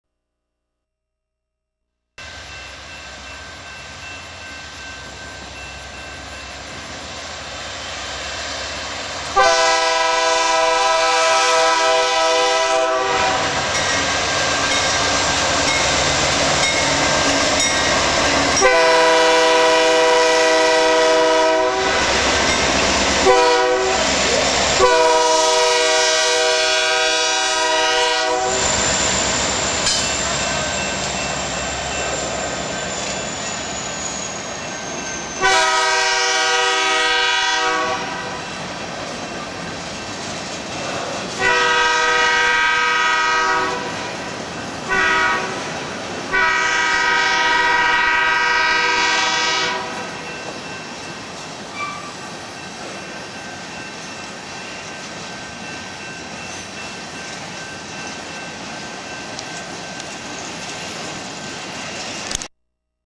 CN 2457 Leads X-PEIEDU
CN 2457 and 8831 pull the X-PEIEDU empty grain train WB through Elmwood, IL on August 15, 2008.